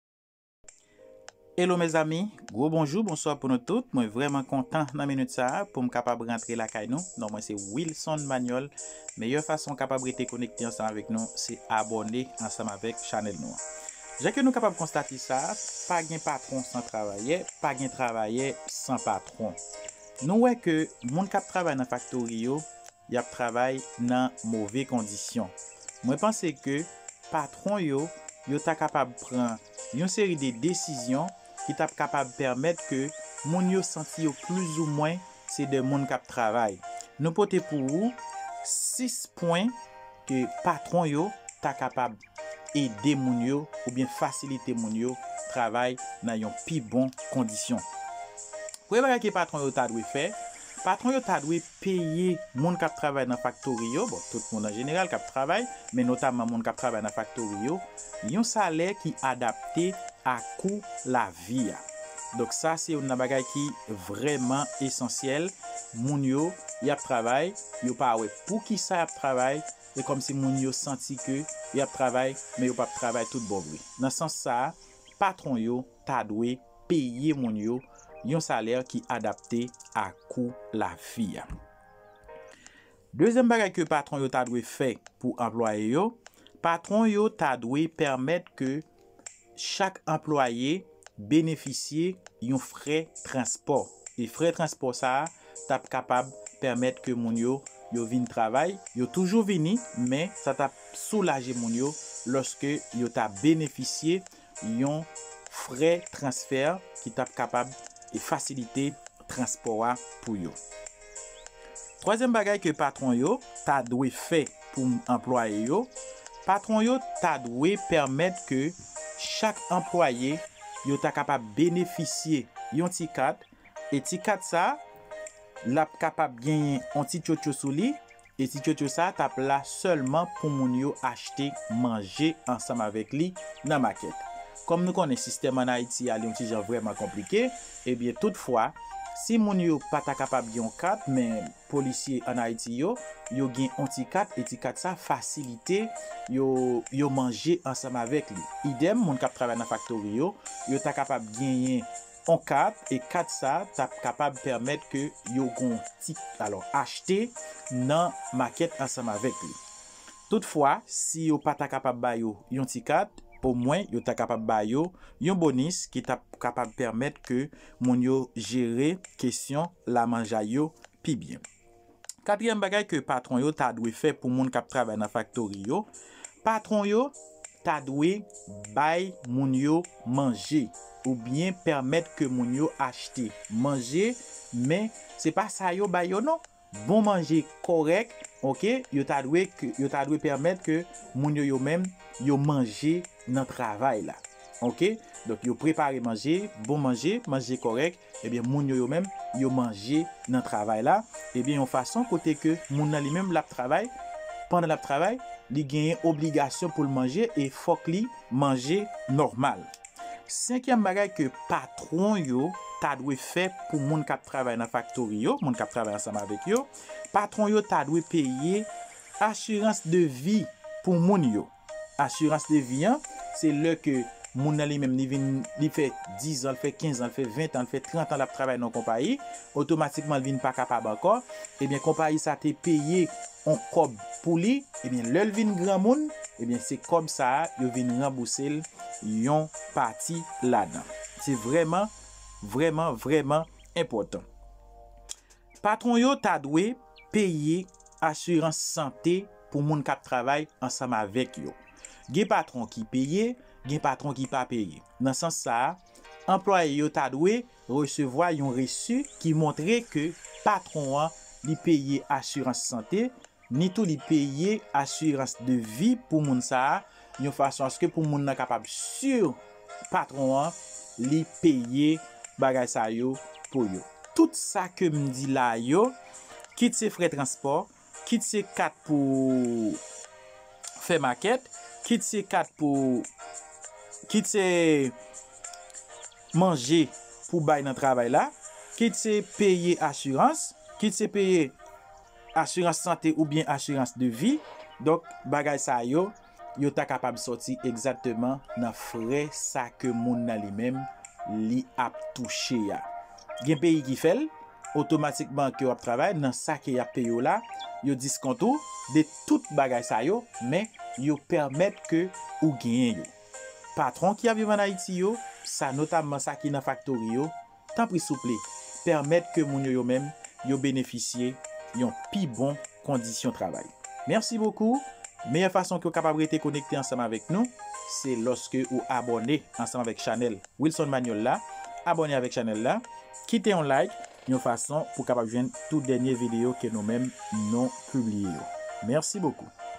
Men koman lavi moun kap travay nan faktori an Ayiti yo pral chanje.
Moun kap travay nan faktori an Ayiti yo merite yon pi bon kondisyon travay.